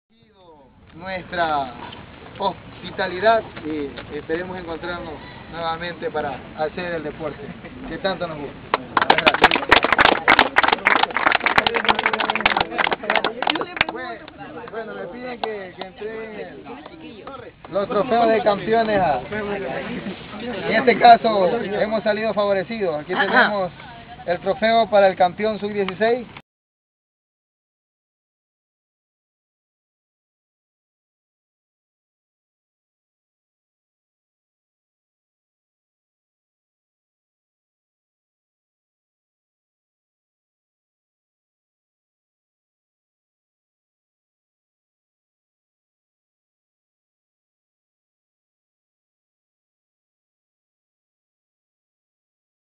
ISRAEL SPORTING EN VINCES, Premiacion a los Campeones.
Momento en que se premia a los campeones del evento disputado en VINCES, en su estadio.